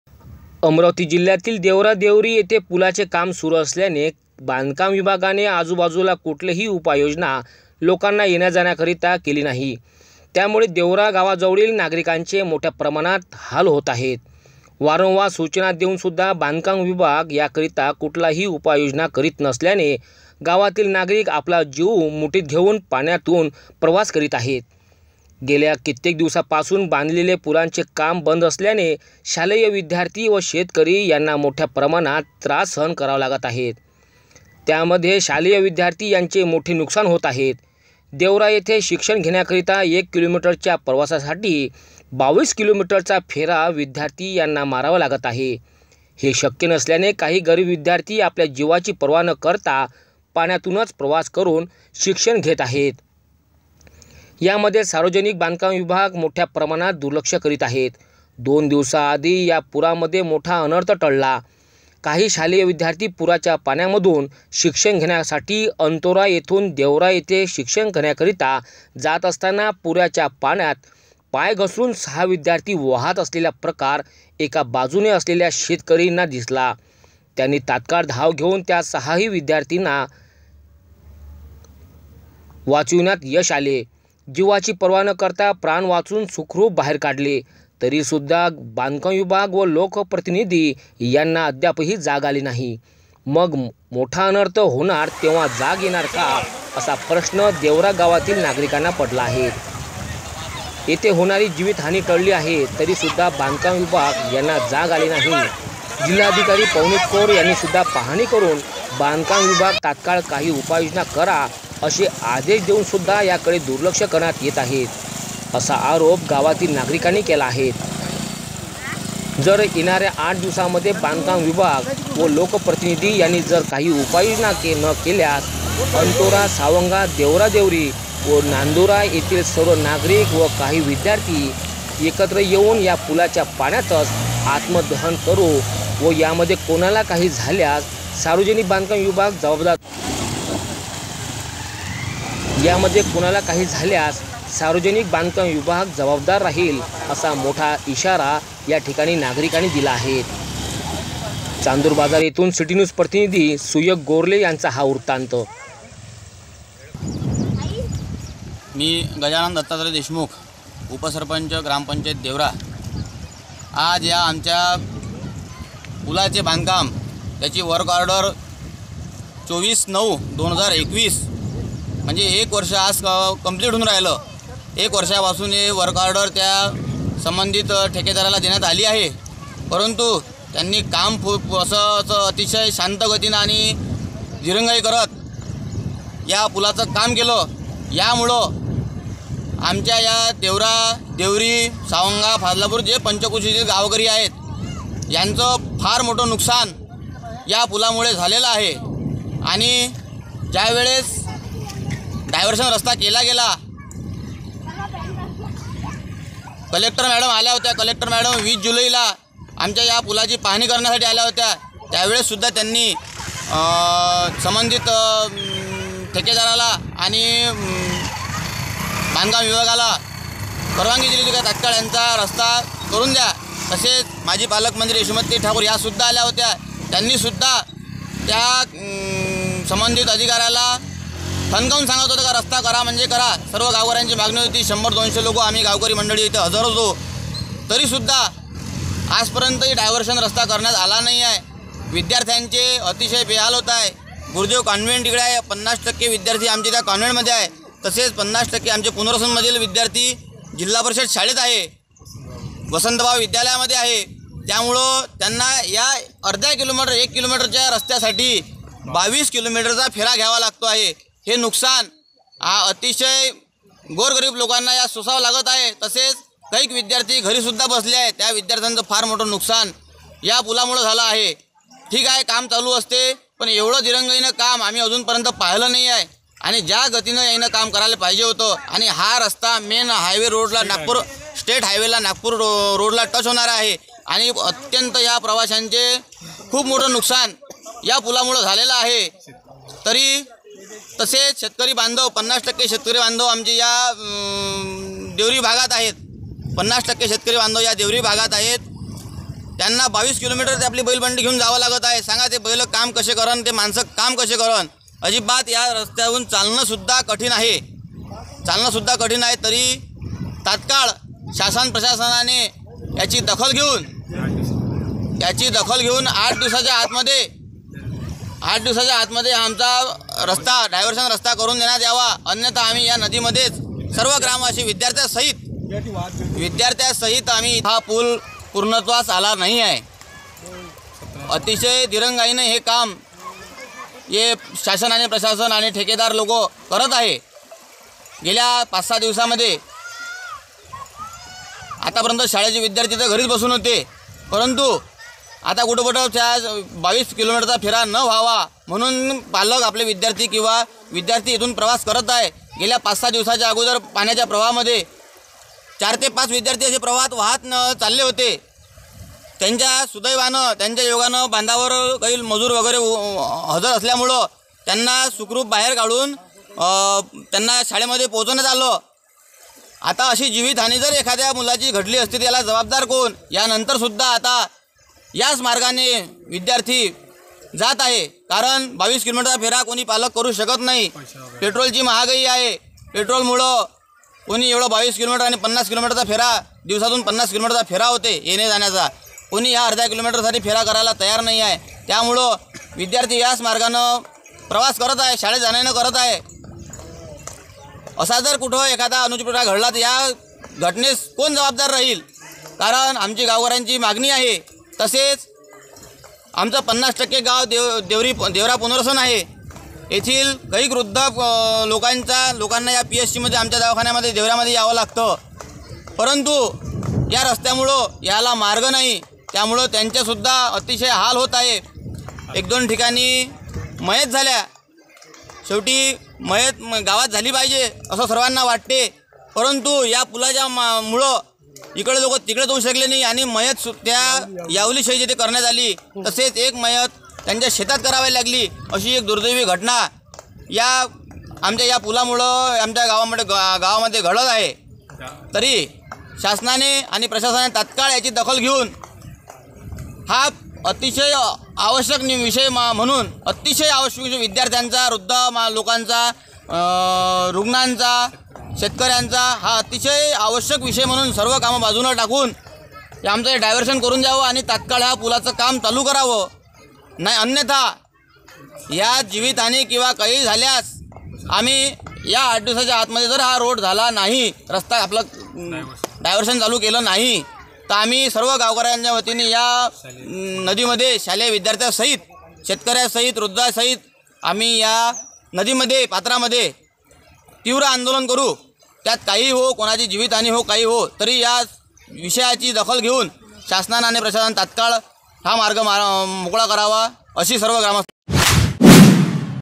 देवरा देवरी येथे पुला मध्ये बांधकाम विभागाचा भोंगळ कारभार
पुला बाजूने कुठलीही उपाय योजना नसल्याने  गावकरी वृद्ध नागरिक, विद्यार्थी त्रस्त
पुलामध्ये पाणी जास्त असल्याने 1 किलोमीटर पाण्यातून न  जासाठी 22 किलोमीटर फेरा
आजूबाजू गावतील शालेय विद्यार्थी पाण्यातून प्रवास करून  देवरा येथे घेत आहे शिक्षण
जिल्हाधिकारी,व लोकप्रतिनिधी यांनी पाहणी करून सुद्धा  दिलेल्या  आदेशाला  बांधकाम विभागाने दाखवली केळाची टोपी
9 FOR BULETIN WHATSAPP MASSEGE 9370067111#citynews_amravati